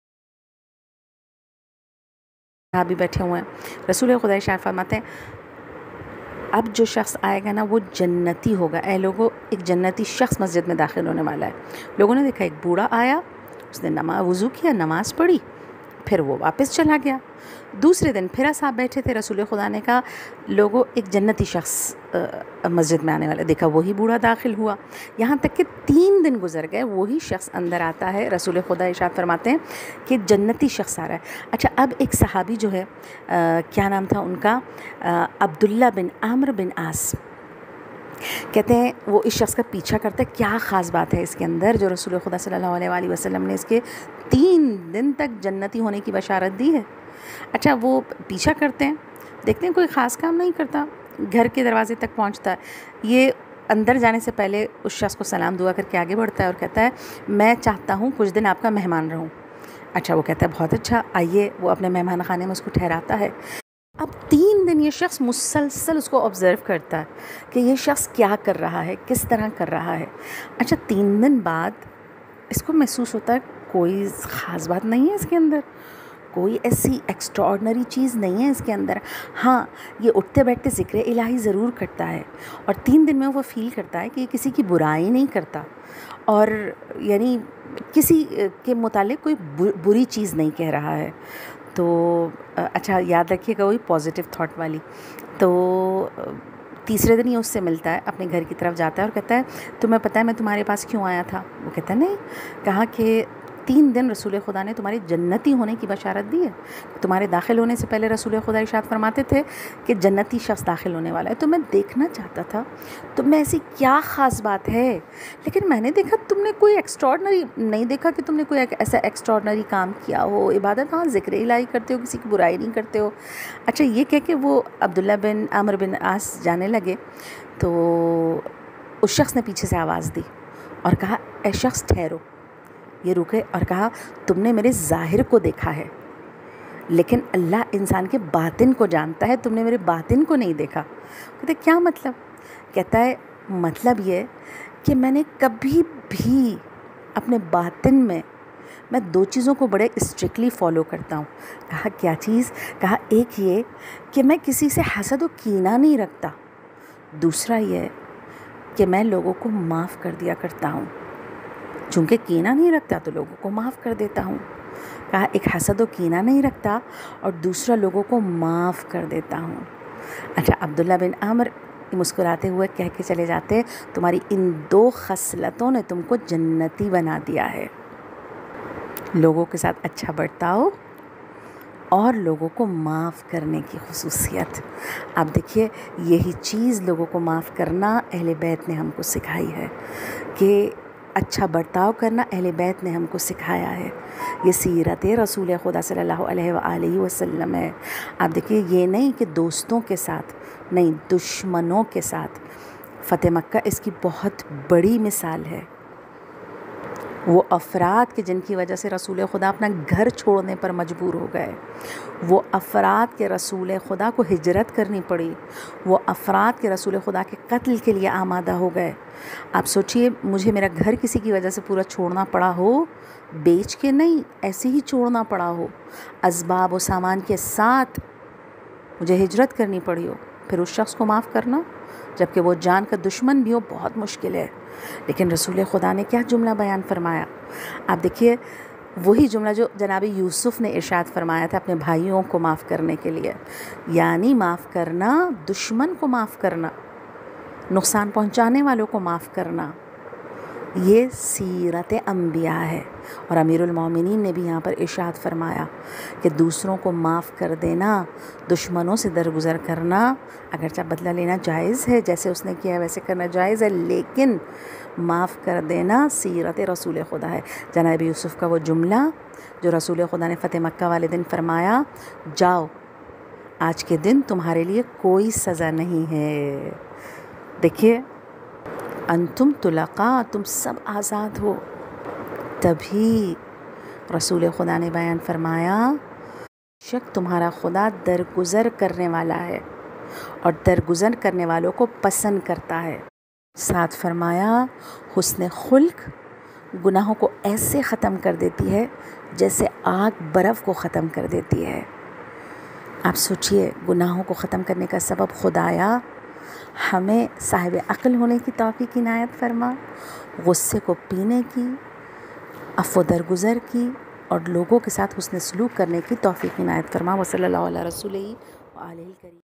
How to Control Anger part2|By Zakira Shama Anjum|غصّہ کیسے کنٹرول کریں بزبانِ ذاکرہ شمع انجم صاحبہ
What is Anger and How to Contril it?
according to imam saying By Zakira Shama Anjum